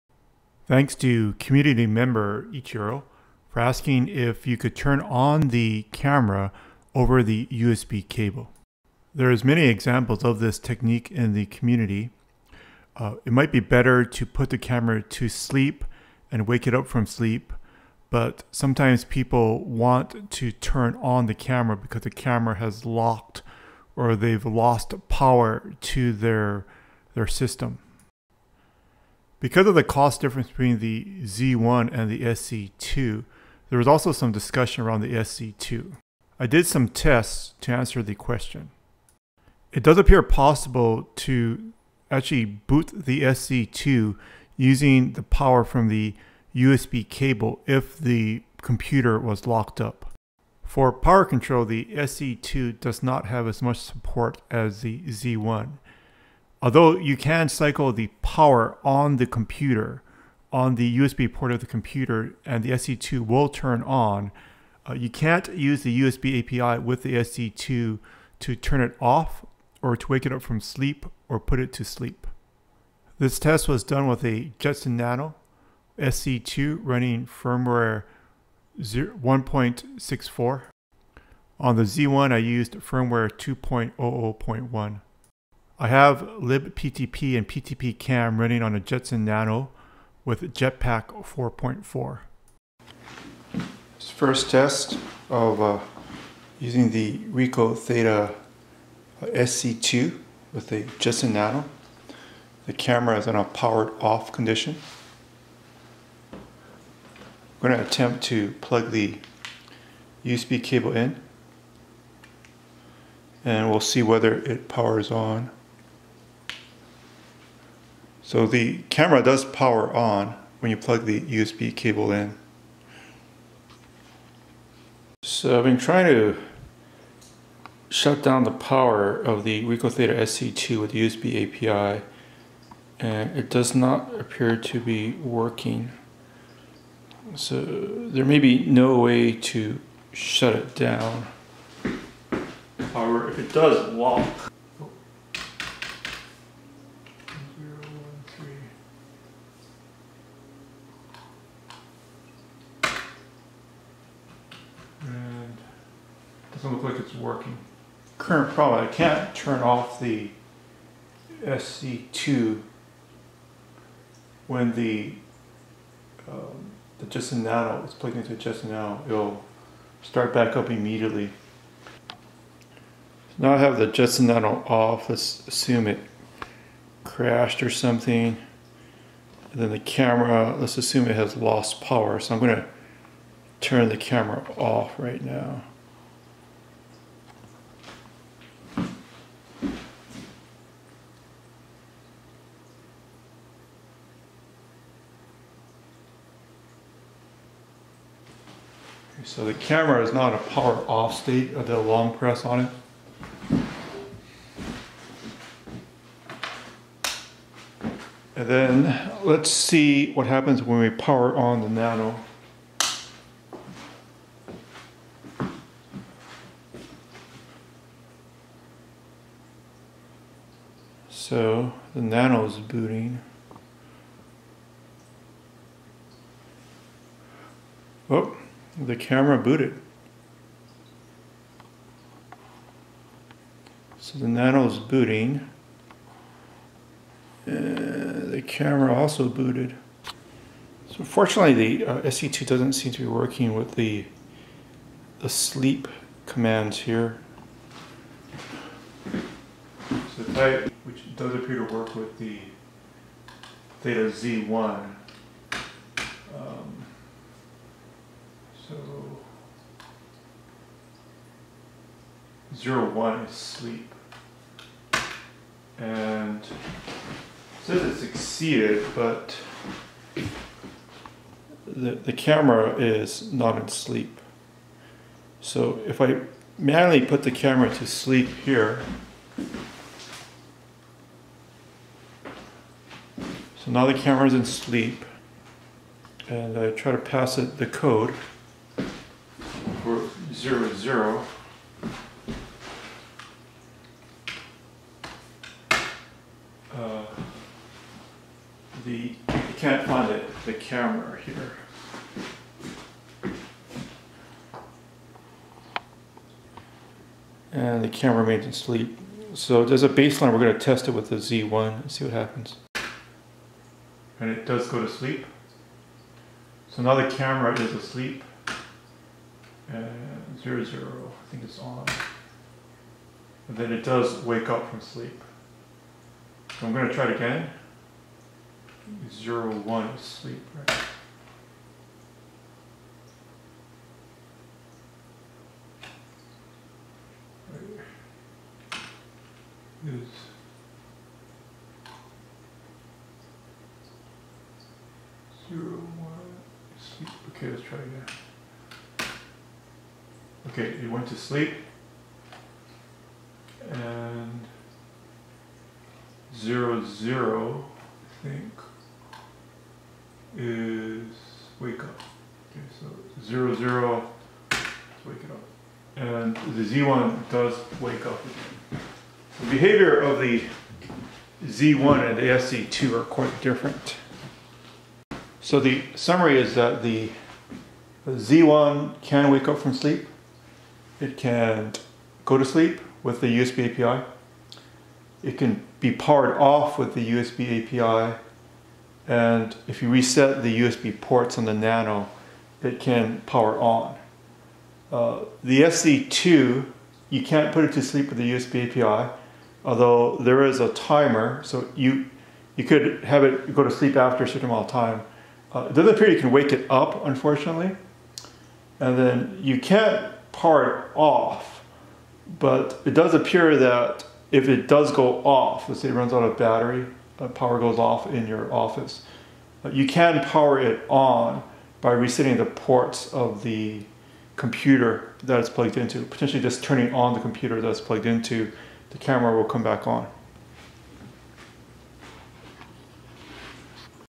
RICOH THETA SC2 and Z1 Power On Tests for Remote Monitoring with 360 Video
Community tests on the RICOH THETA Z1 and SC2 to see if the cameras can wake up from sleep or be powered on using a USB cable.  This is applicable for a wide range of remote monitoring systems for construction, environmental research, hazardous site monitoring.  The common use case is to connect the RICOH THETA to a Jetson Nano or a Raspberry Pi with a USB cable.  The cable can supply the camera with power and you can run commands over it.
There is a large amount of community content on the USB API for RICOH THETA 360 cameras

If you are interested in the USB API for RICOH THETA 360 cameras, there is a series of videos here: